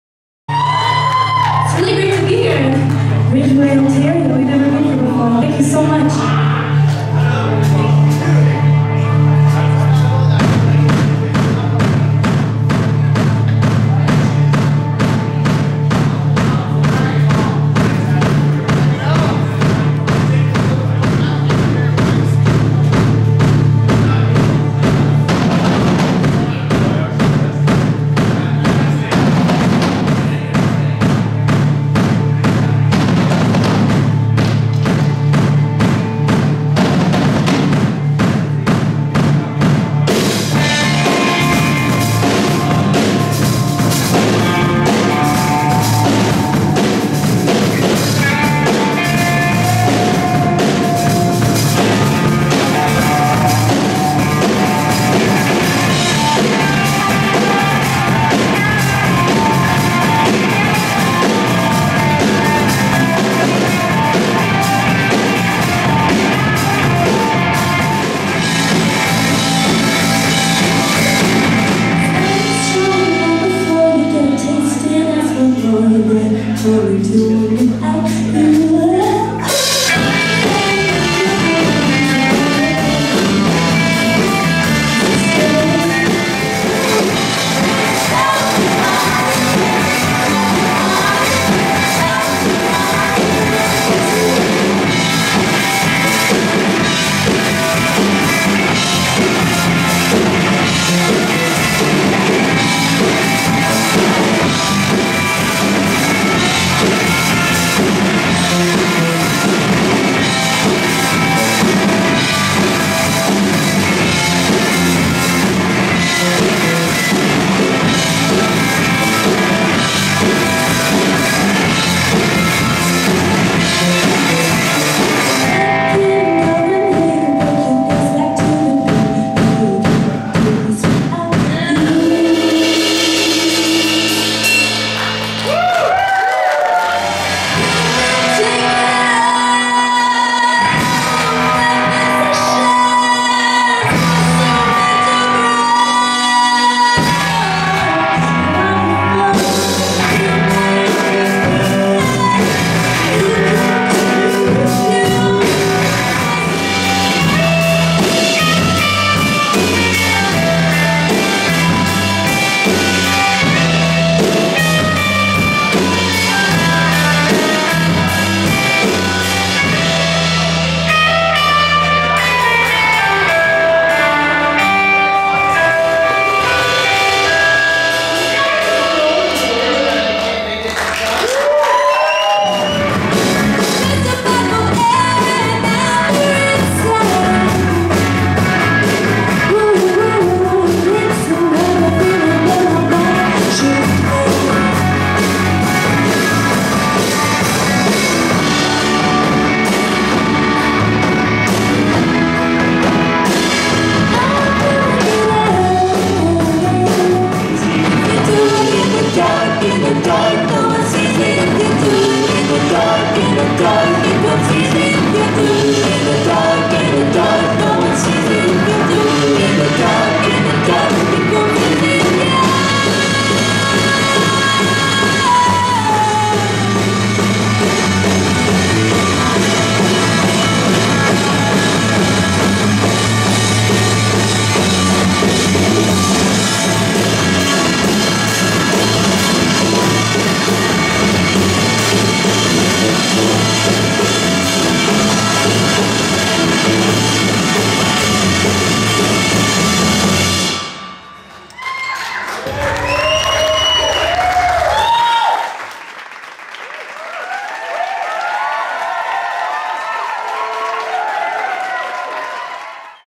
The Balconies Live play The Sanctuary, Ridgeway
The Balconies delivered a high energy set at the Sanctuary in Ridgeway.  February 2012.  Check them out on FBook.
The Sanctuary is located in Ridgeway, Ontario and is a vibrant arts centre and concert hall.